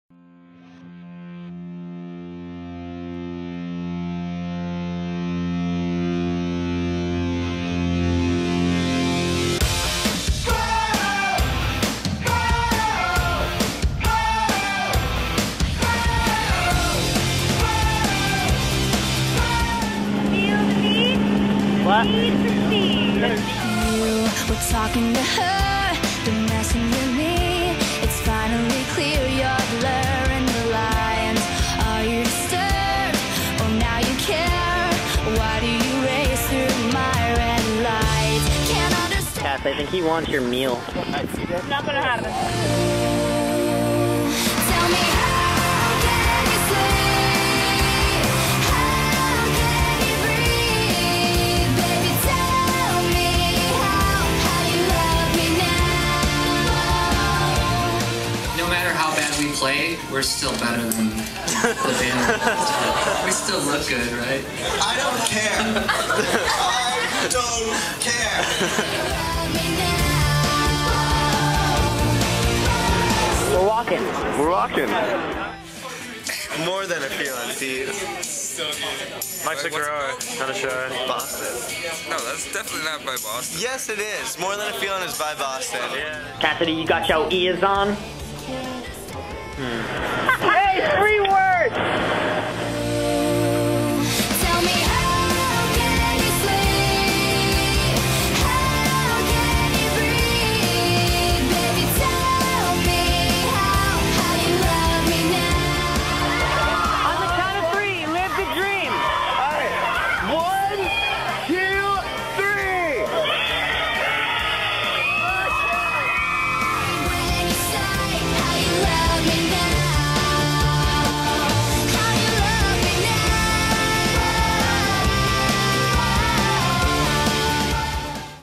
Footage From World Tour 09
Footage from Hey Monday's recent world tour, which included stops in Japan, Singapore, Manila, Australia, and New Zealand!